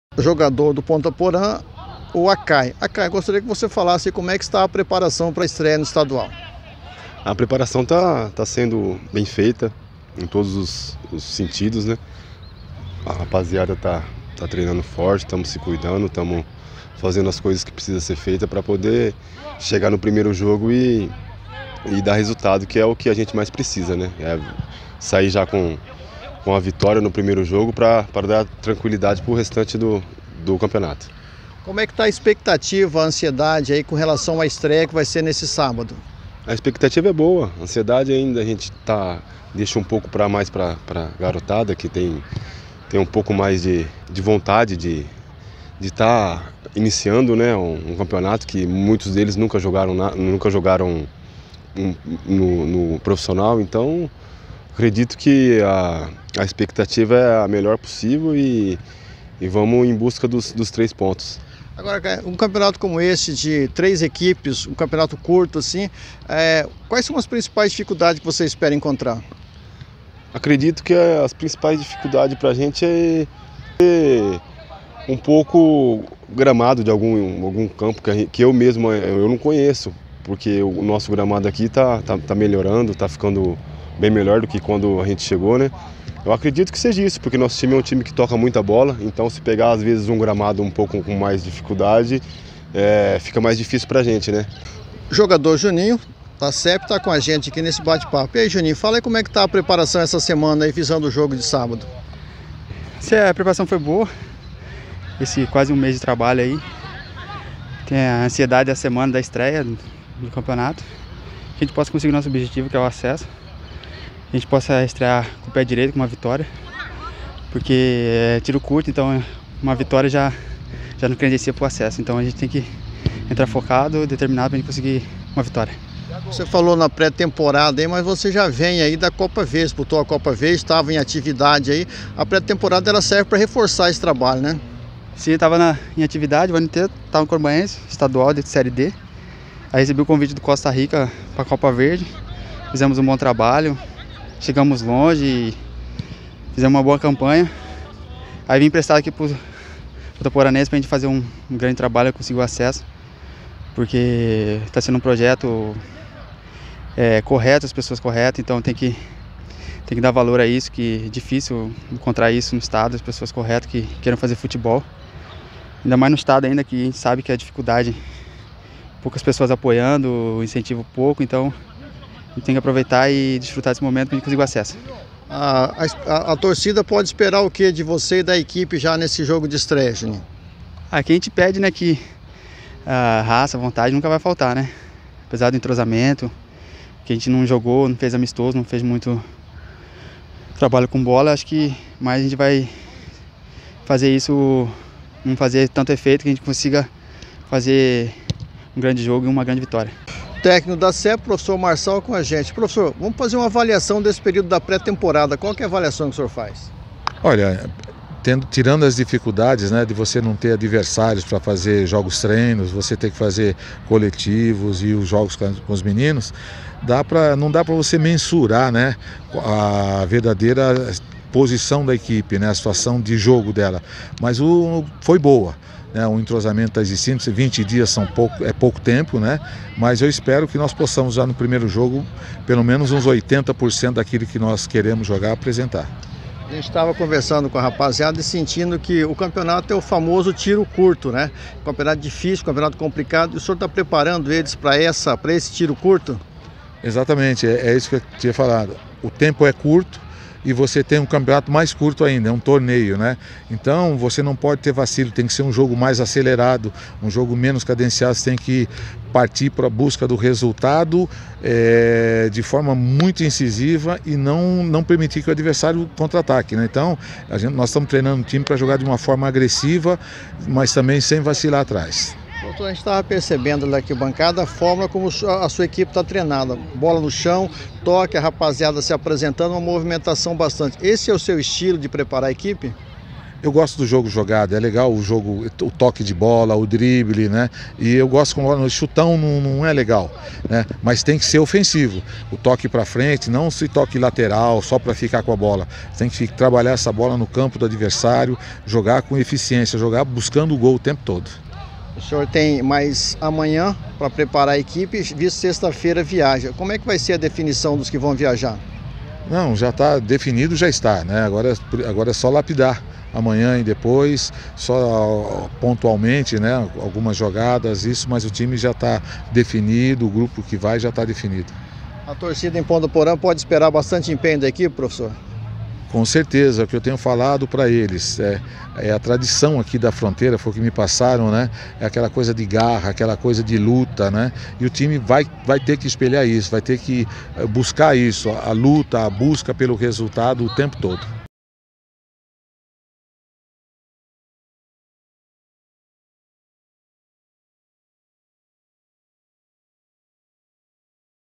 TV WEB: Entrevista com SEP
O elenco da SEP (Sociedade Esportiva Pontaporanense), que já foi uma equipe tradicional do futebol profissional do Mato Grosso do Sul, sendo que na década de 90 chegou a ser vice-campeã do estado, e disputando uma fase da copa do Brasil, retornou com força para disputar o campeonato estadual da segunda divisão.

A Federação de Futebol bem que poderia poupar os dirigentes de participar de um evento tão sem atrativo como o que foi programado, uma vez que das oito equipes que participaram da primeira reunião para tratar da segunda divisão, ficaram apenas três, isso mesmo, apenas três equipes que estarão fazendo de tudo para garantir a sua presença na primeira divisão do próximo ano que começa já no mês de janeiro de 2020, sendo que se classifica apenas dois clubes para a primeira divisão do próximo ano.